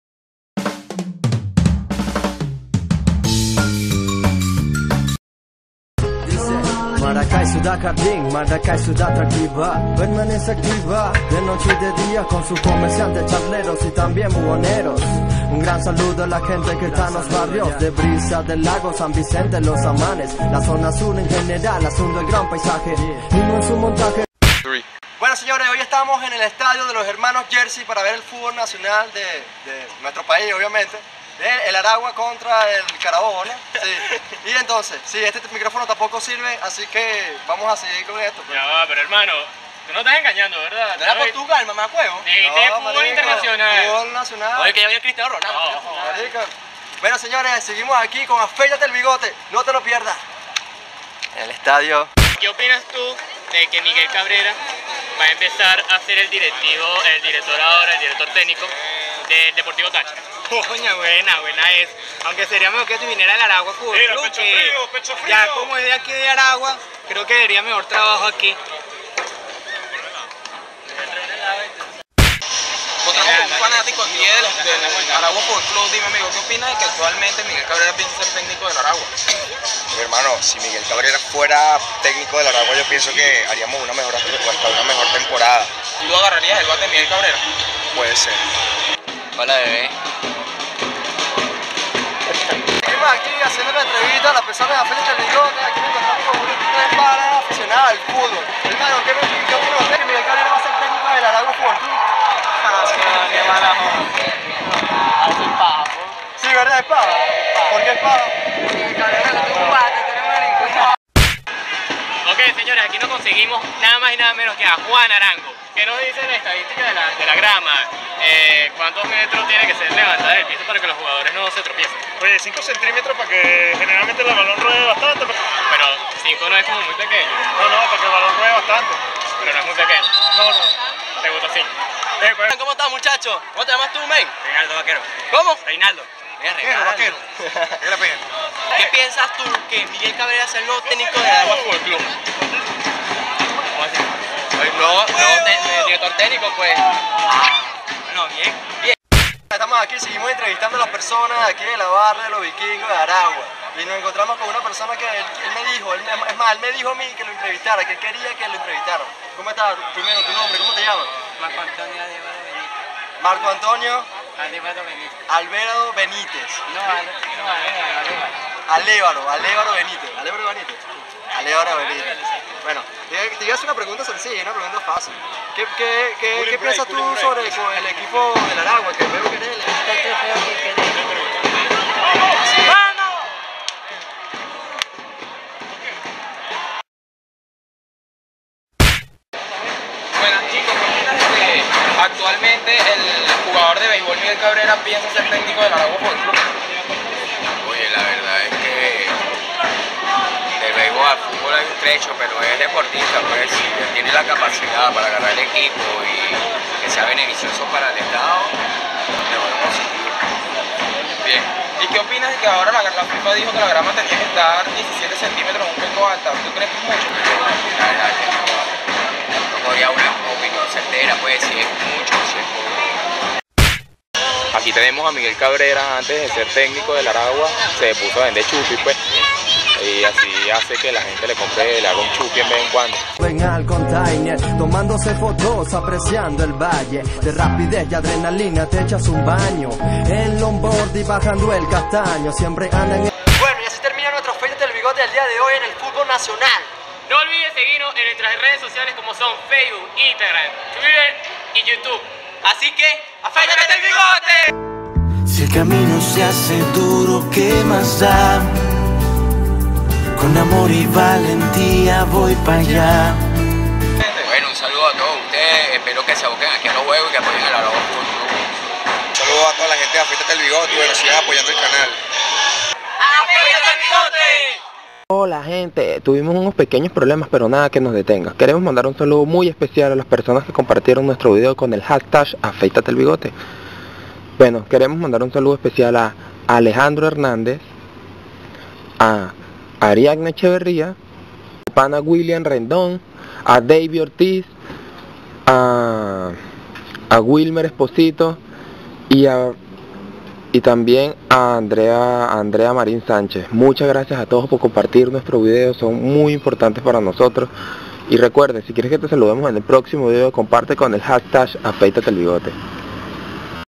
JUAN ARANGO En el Estadio hermanos Ghersi |AFEITATE EL BIGOTE
Like por el ARAGUA F.C,  el bate de Miguel Cabrera y por Juan arango que estuvieron en el juego apoyando